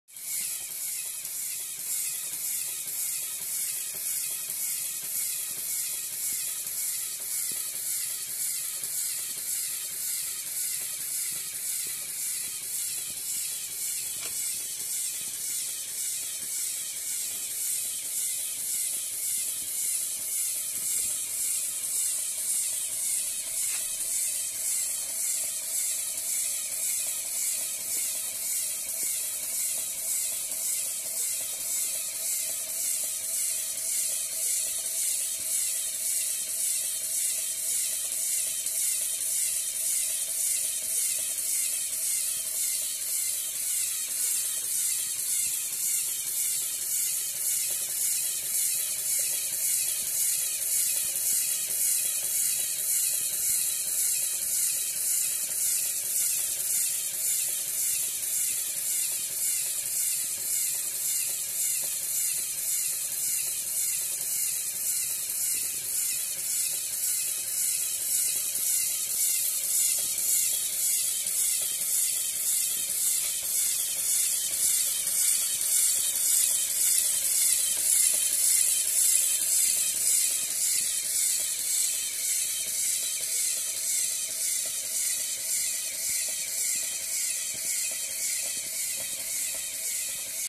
LEGO Aquarium with Technic and Power Functions - v2
New:
- bubble curtain
- attempt to hide cables and hoses
- 4-pump Sariel-like compressor
- independent power supply for the ligths
- style rearragement
This is the final version, at least for now. It will be shown in PLUG' exhibition next week at Braga BRInCKa 2016.
Me and my wife are very pleased with this project and with people reactions to it. What started just as an experiment is now a very funny and educational MOC.